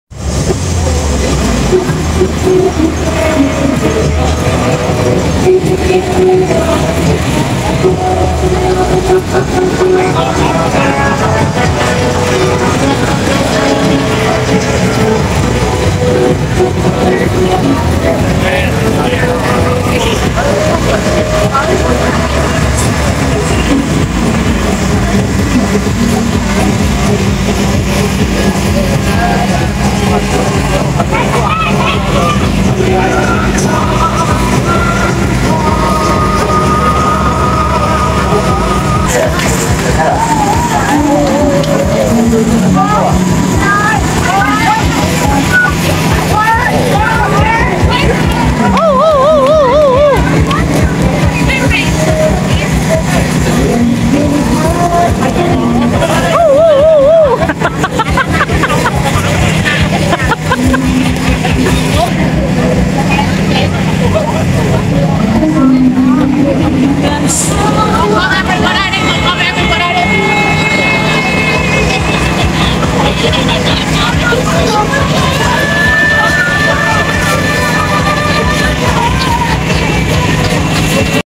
Biển Nha Trang lúc này đang diễu hành liên hoan du lịch
Biển Nha Trang lúc này đang diễu hành liên hoan du lịch#kenhdulich